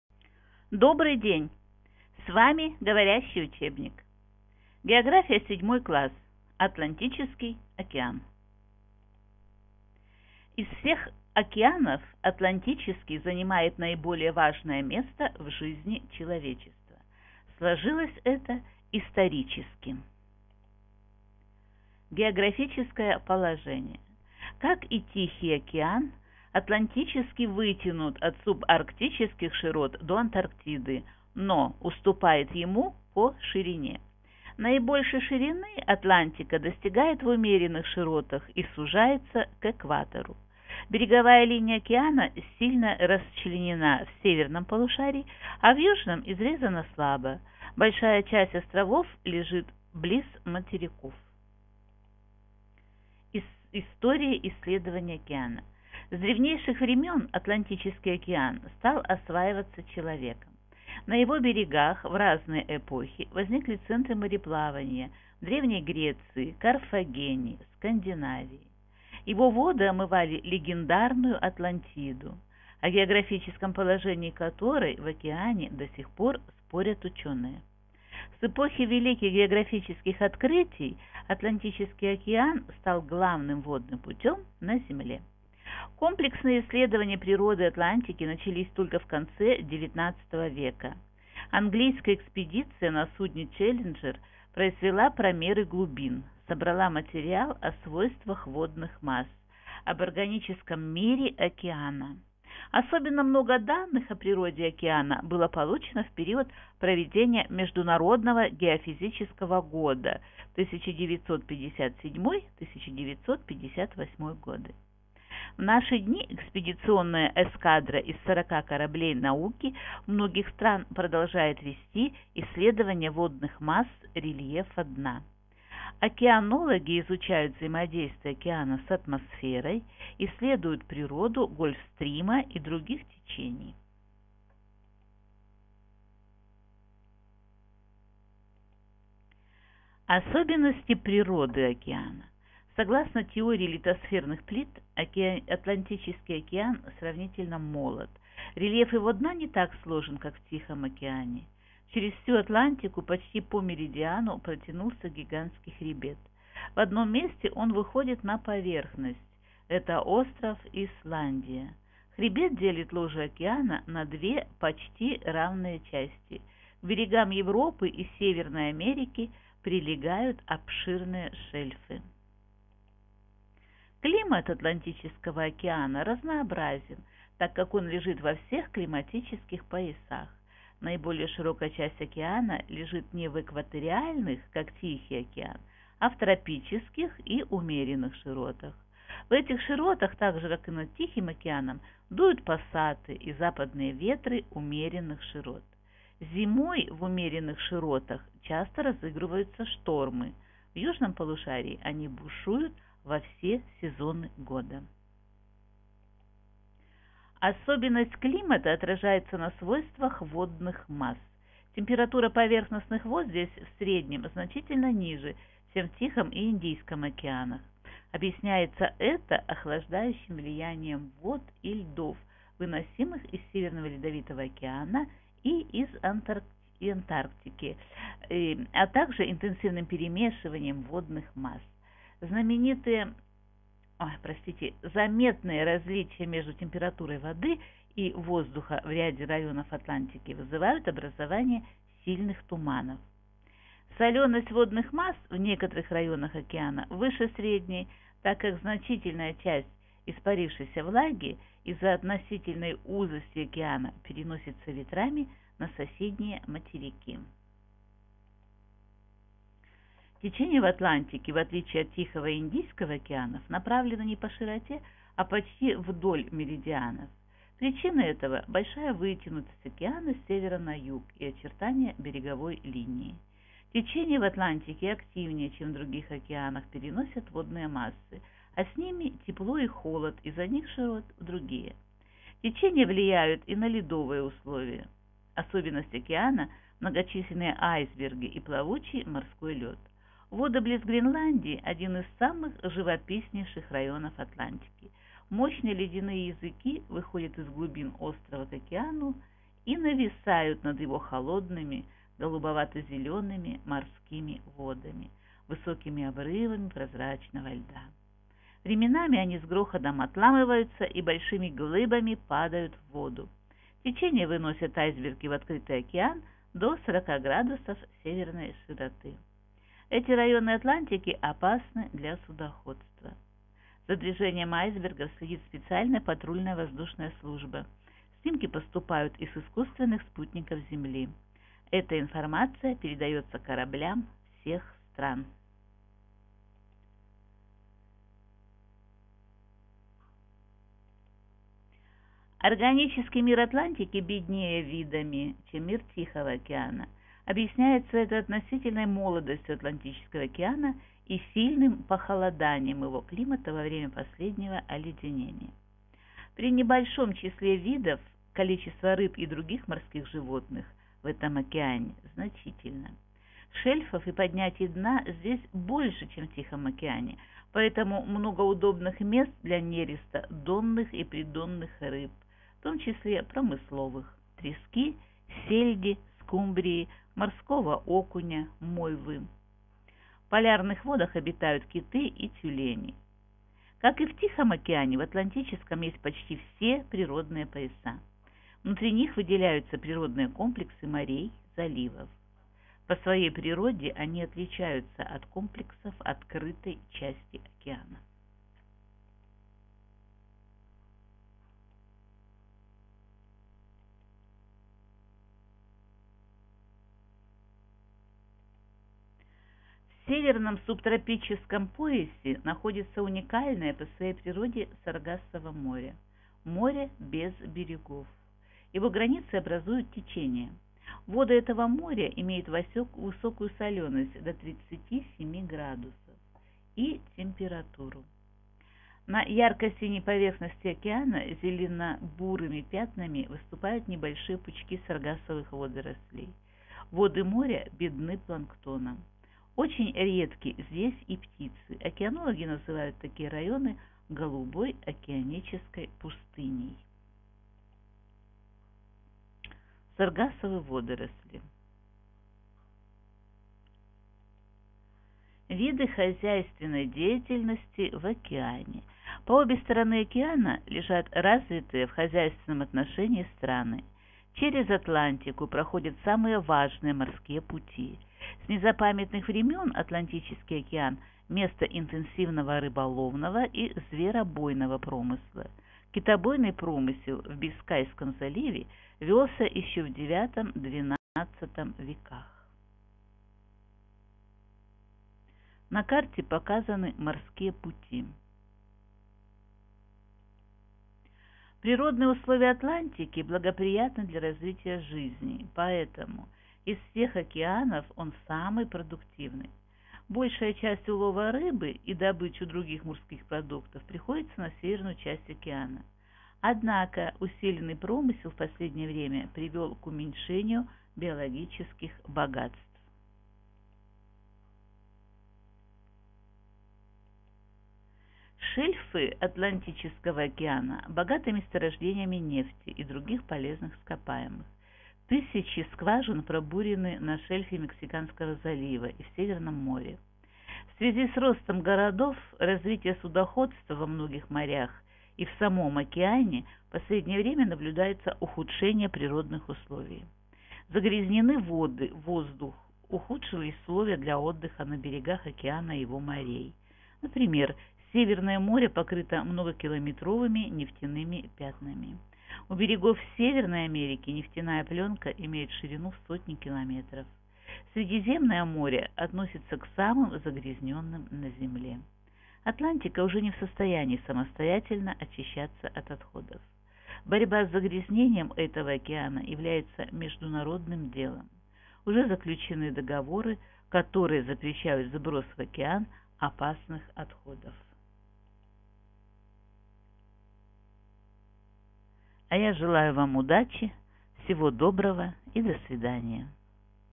Атлантический океан. География 7 класс
Из всех океанов Атлантический занимает наиболее важное место в жизни человечества. Сложилось это исторически.